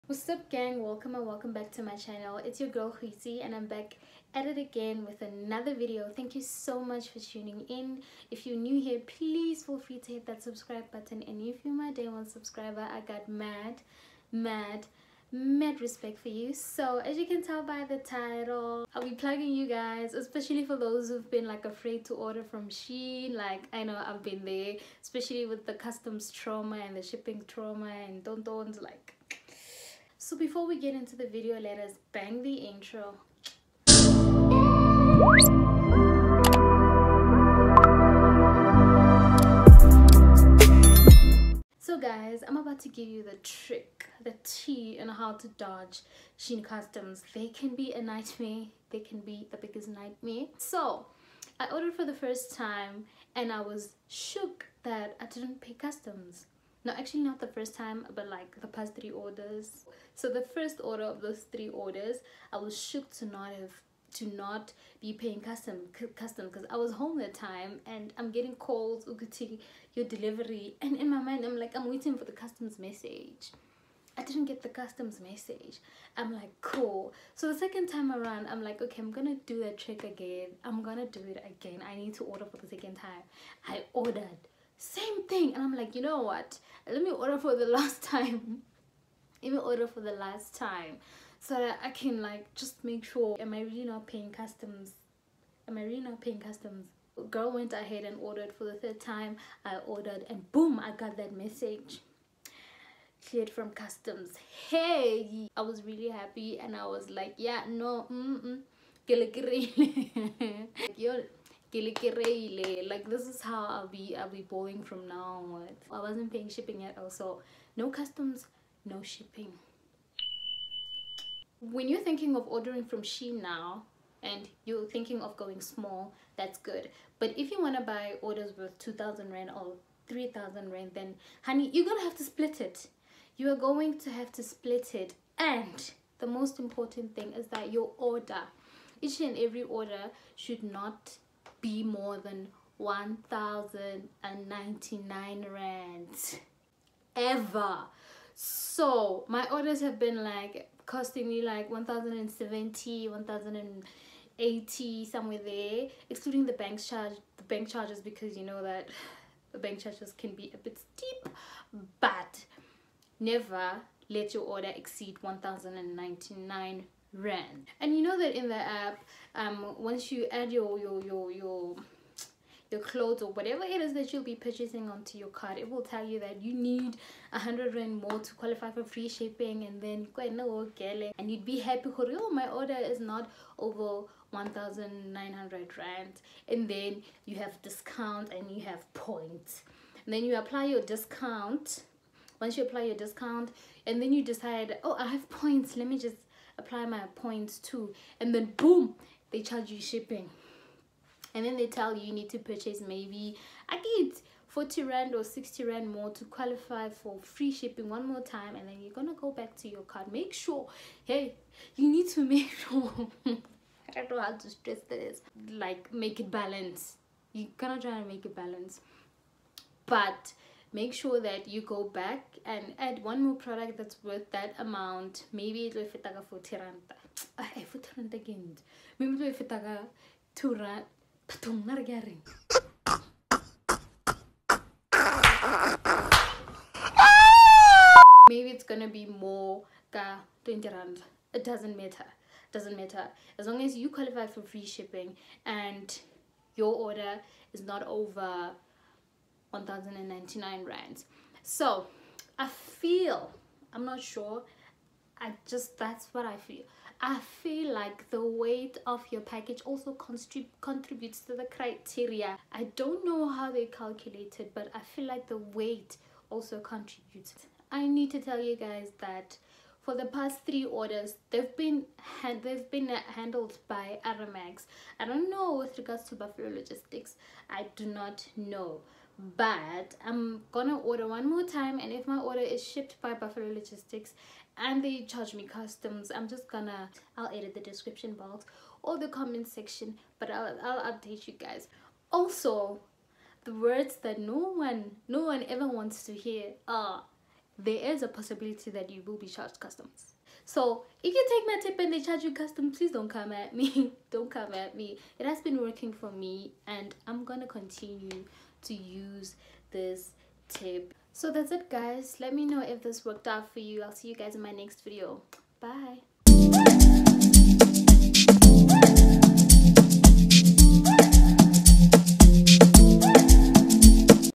AVOID SHEIN CUSTOMS SIS! (ditch them) | South African Youtuber
Hey everyone, hope y'all are alive and well. Welcome or welcome back. I hope this 5 minute video helps you with regards to the SHEIN customs and shipping. Thank me later!

Goal: 1k

Timestamps
0:00 Intro
0:42 PLUG
05:11 Outro